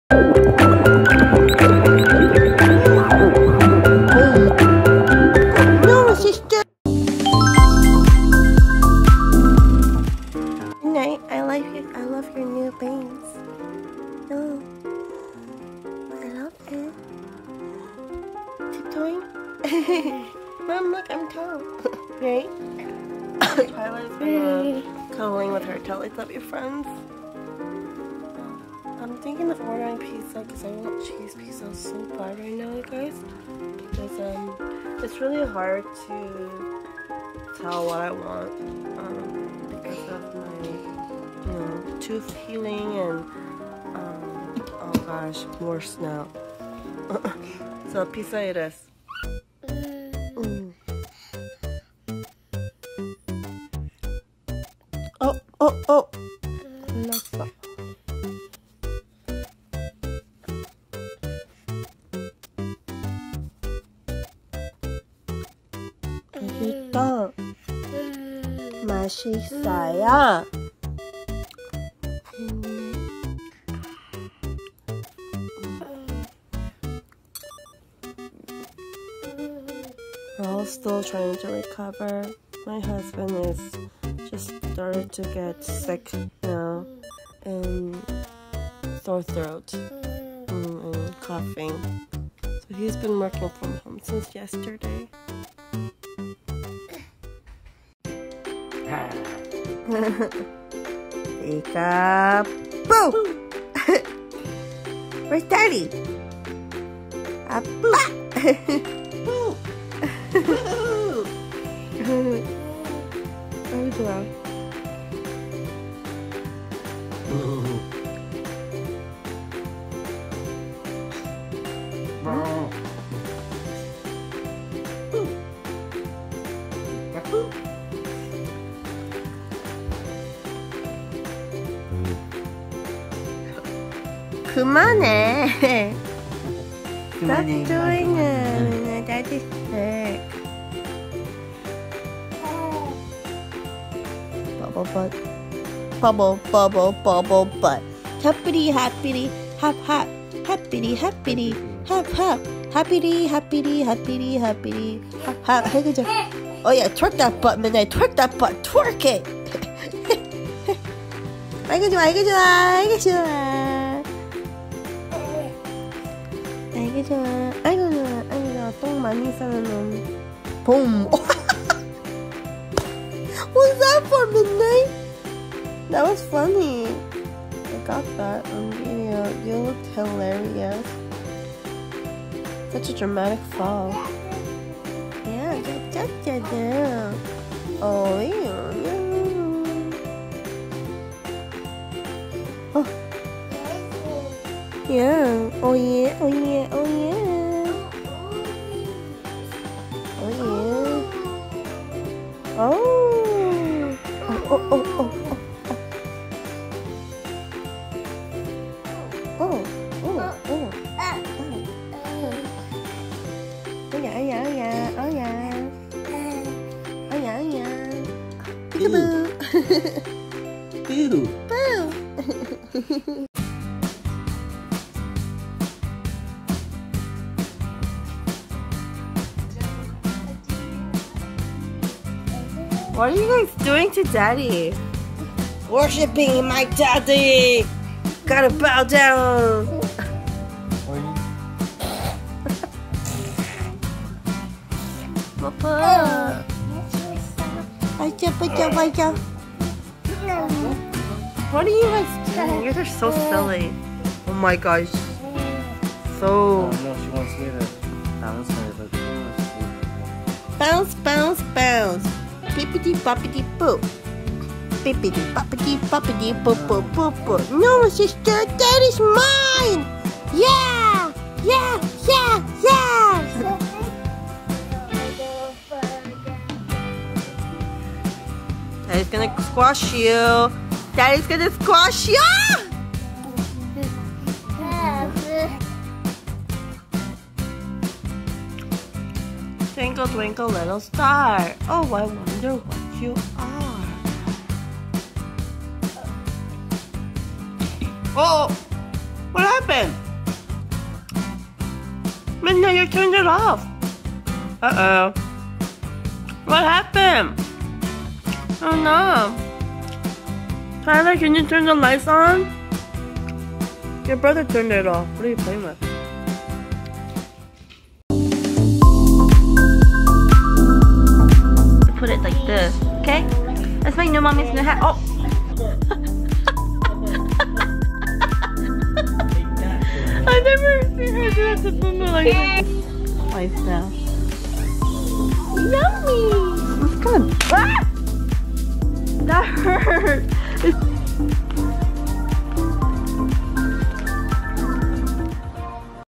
11132019: Clingy to their daddy | Vlog #2154
❐ VIDEO TIMELINE ❐
♦ We're all sick
♦ Midnight playing peek a boo with his daddy's hoodie
♦ That dramatic fall
♦ Our kids getting all clingy with their daddy

❐ VLOG CAMERAS USED IN THIS VLOG ❐